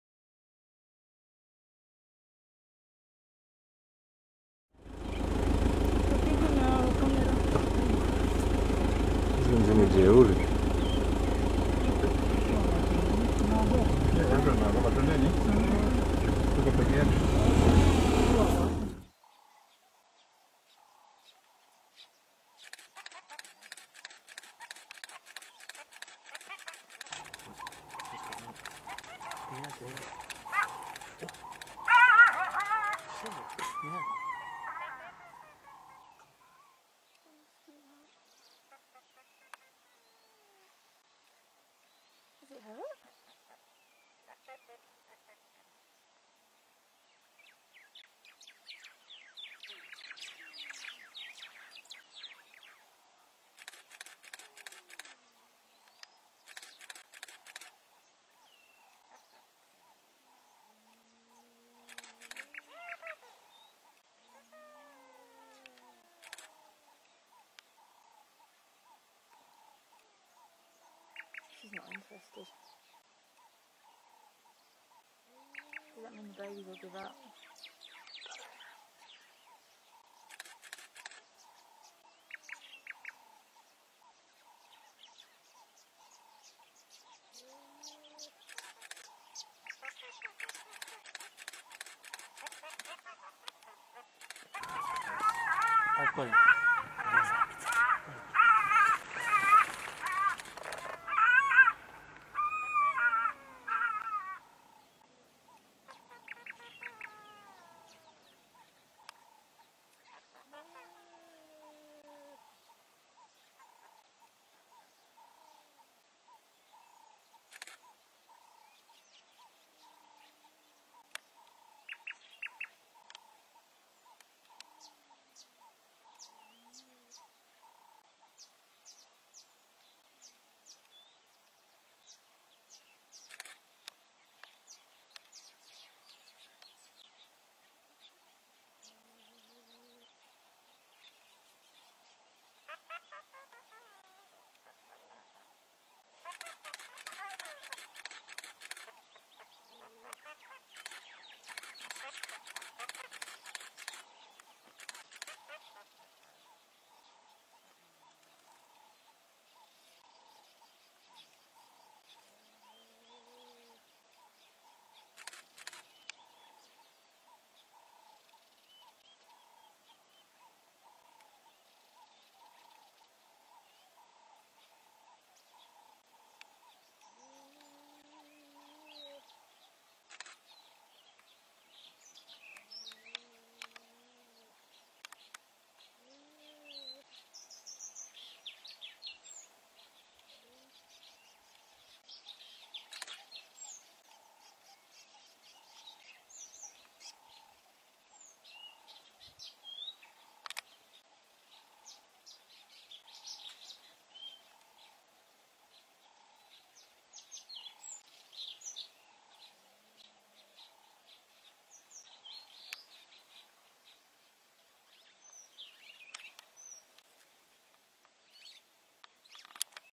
Cheetahs being very aggressive towards a jackal
An unfortunate jackal was confronted by a an aggressive cheetah coalition and got smacked around. Cheetahs are rarely seen this aggressive against another predator. This is because cheetahs despise jackals the same way lions despise hyenas.

Sell your wildlife sightings to Maasaisightings.com